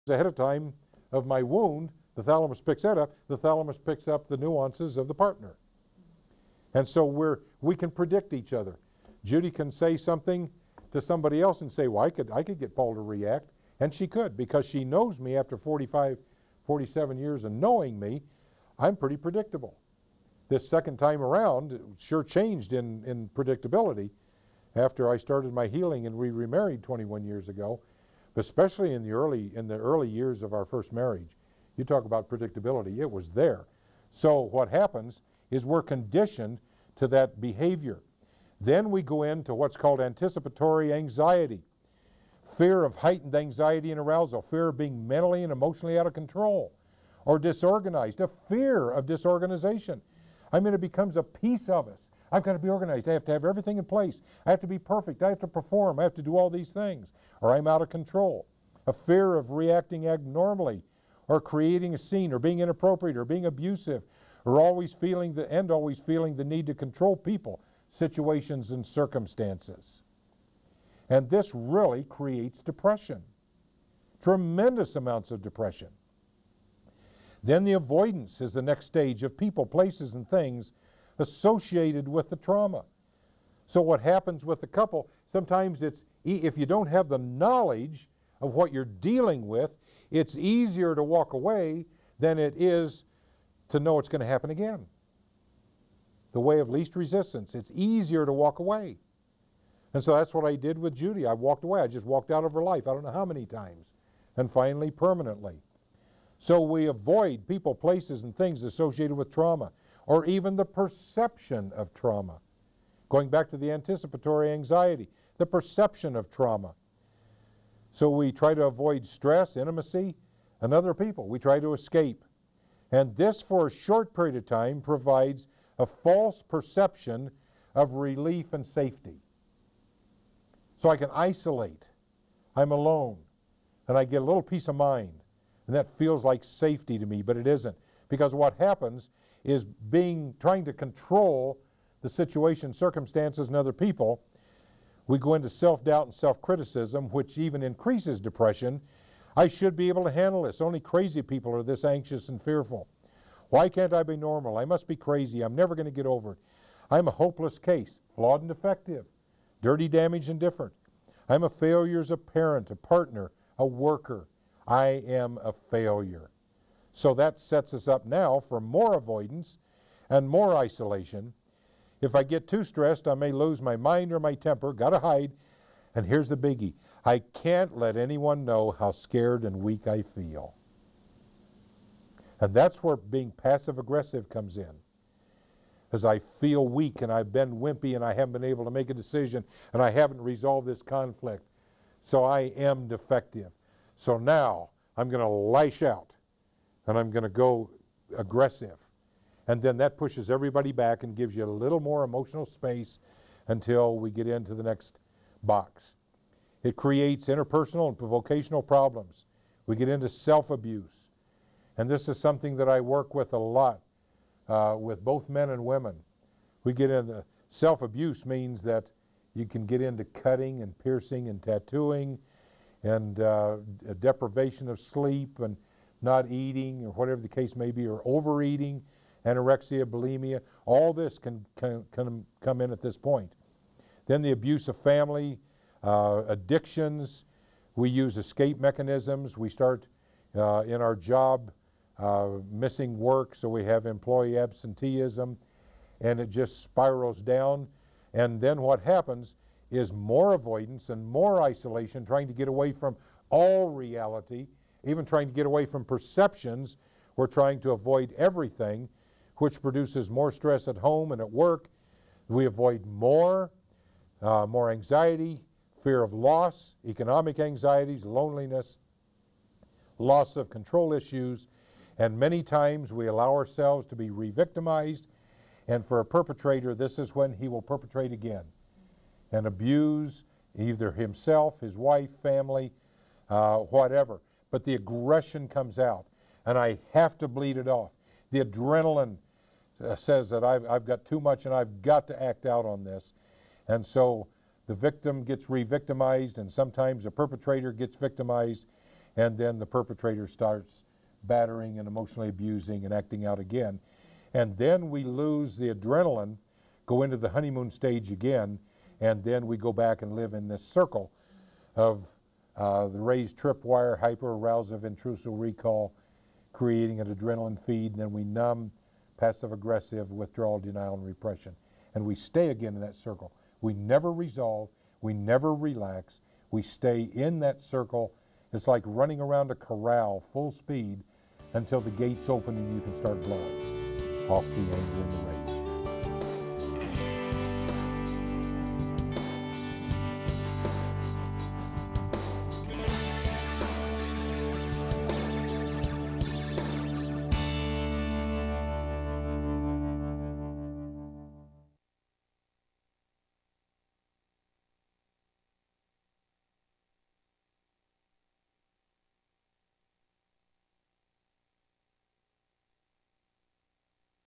Dr, Paul Hegstrom - Brain Science - Lecture 2 Part 3
An insightful lecture on the power of mind maps and how habits form.
Dr. Hegstrom was noted for having overcome alcoholism and domestic violence as he candidly discusses in this talk.